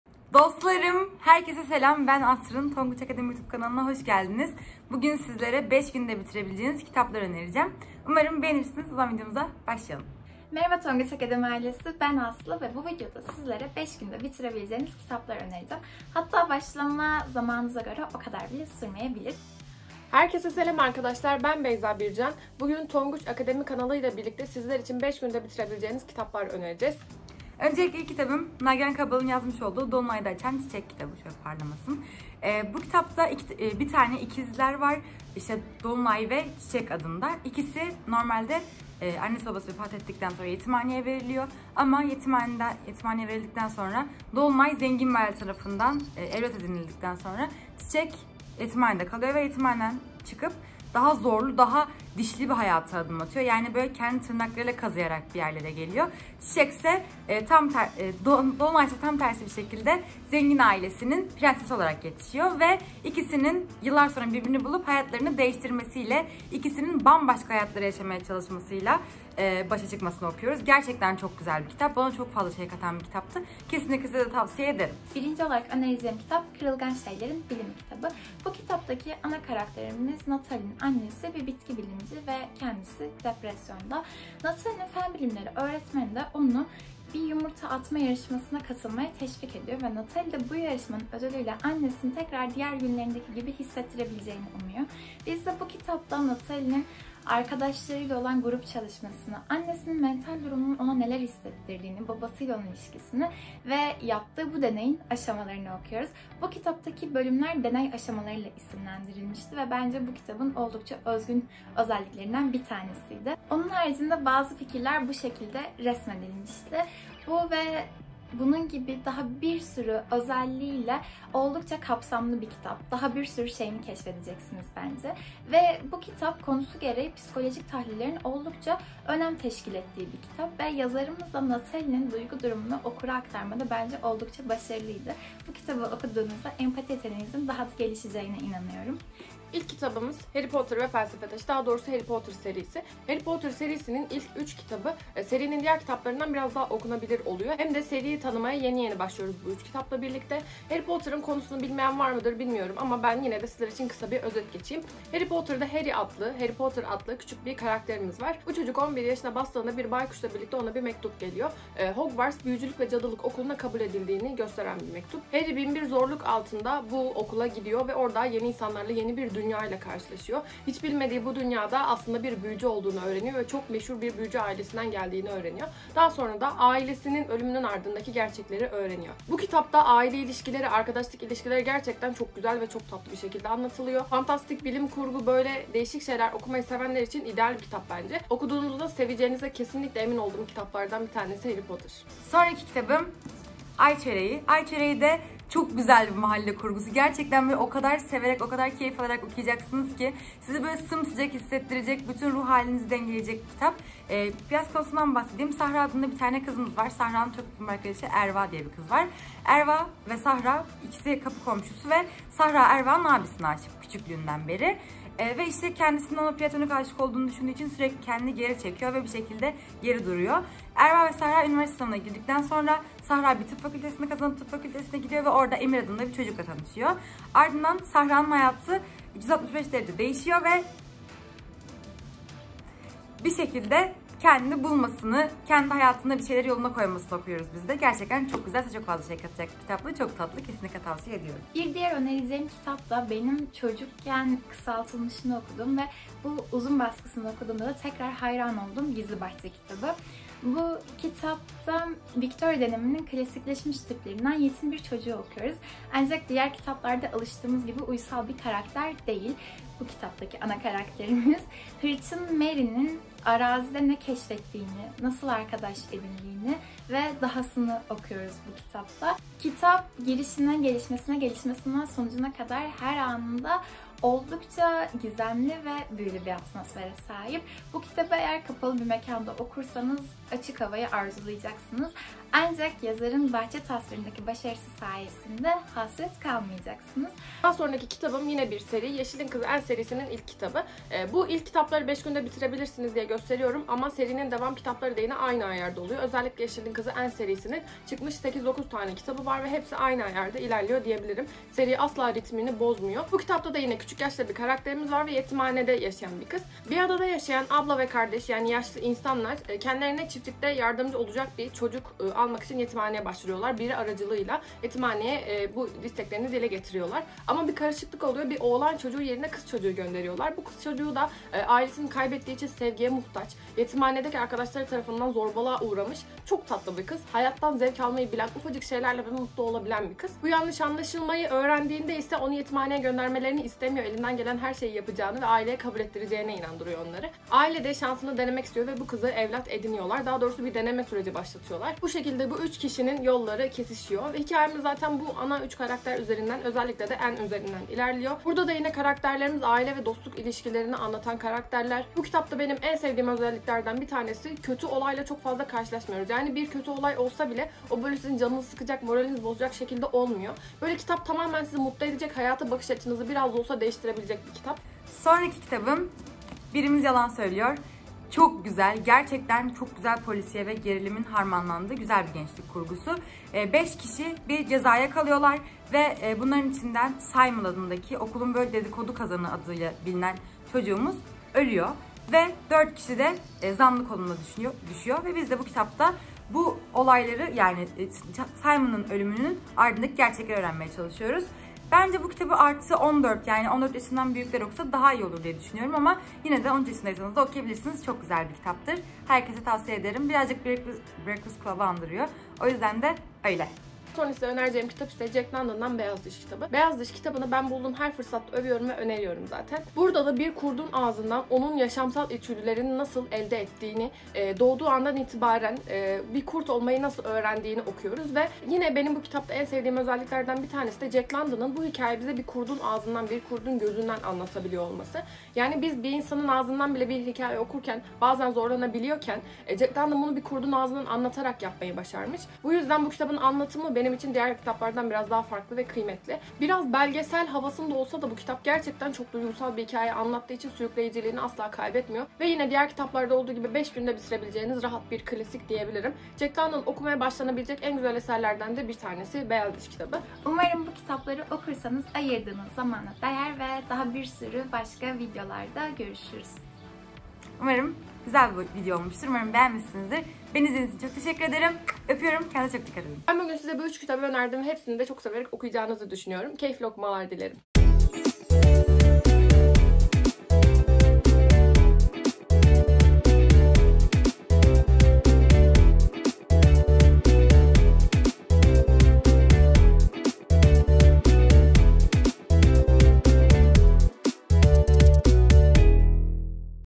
5 Günde Bitirebileceğin Kitap Tavsiyeleri 📚 | Bir Çırpıda Okuyacaksın 😯
Ve 5 günde bitirebileceğiniz kitap challenge yapıyoruz! 🖐 teamfluencer arkadaşlarımız bu videoda okumaktan keyif alacağınız; 5 günde bitirebileceğiniz kitap öneriyor. Bu challenge kaçmaz 😎